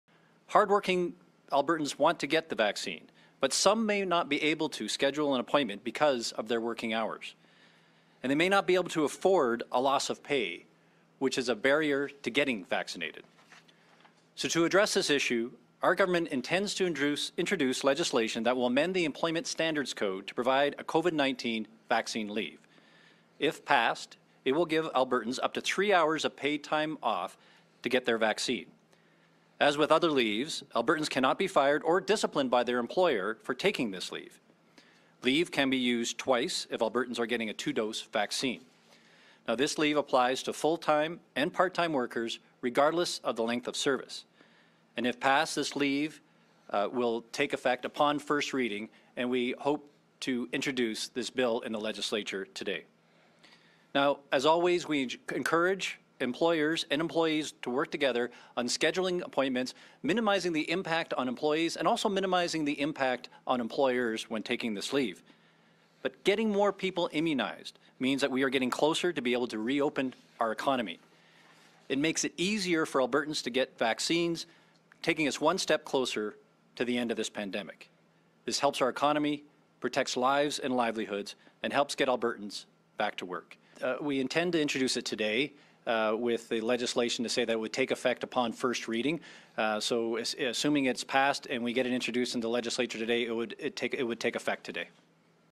COVID-19 : Paid leave for vaccines could become the law in Alberta
Proposed amendments to the employment standards code would provide a COVID-19 vaccine leave and make it illegal for an employer to fire or discipline an employee who takes time off to be vaccinated.